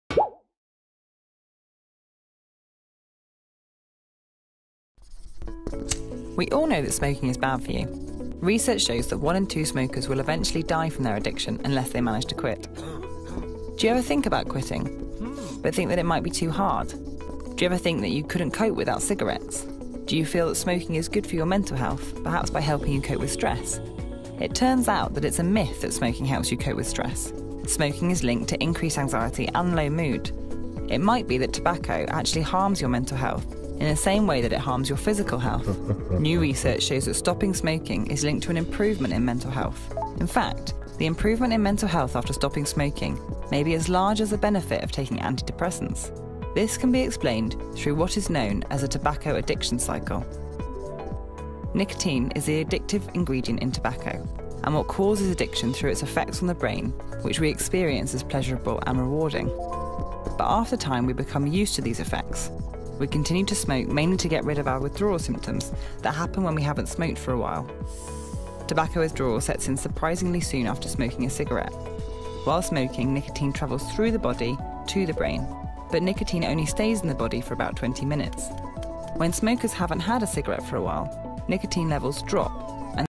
No Smoking Day 2021
Want to know why ex-smokers are happier than smokers? Watch this video.

and start your quit attempt.